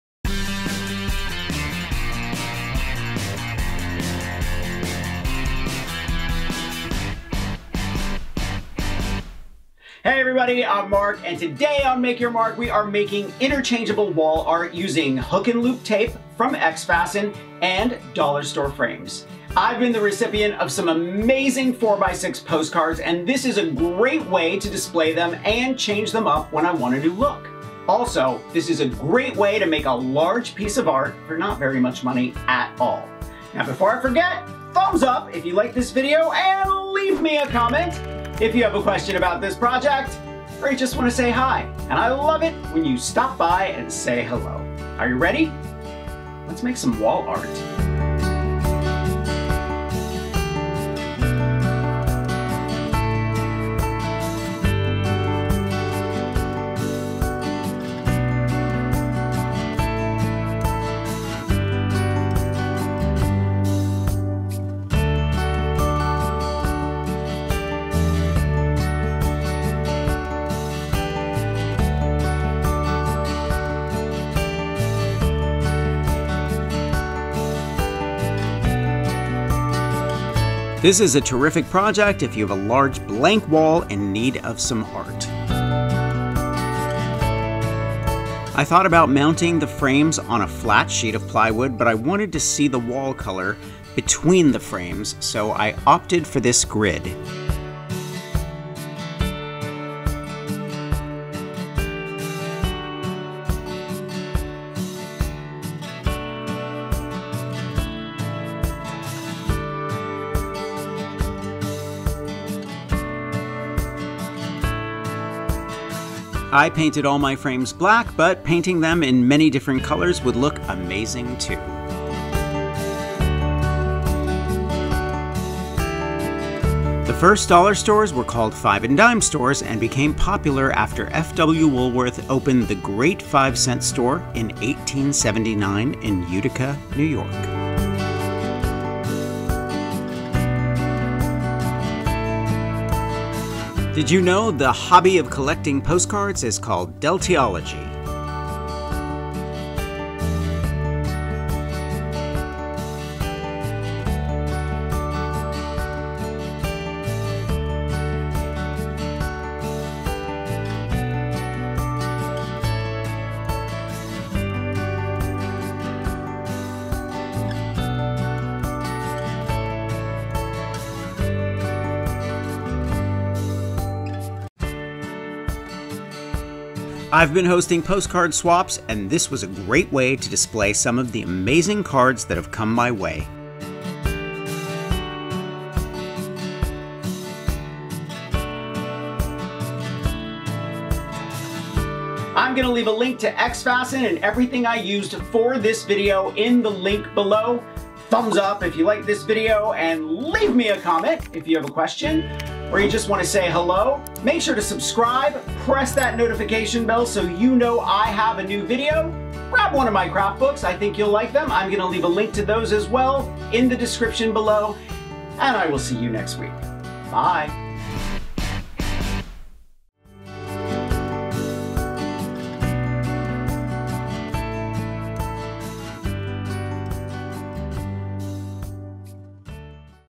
Cluster Frame for Postcard Art or Photos
Turn dollar store 4" x 6" frames into large cluster art to show off your photos or postcards!
XFasten Hook & Loop Tape
Amazing Goop II Max
Black Flat Spray Paint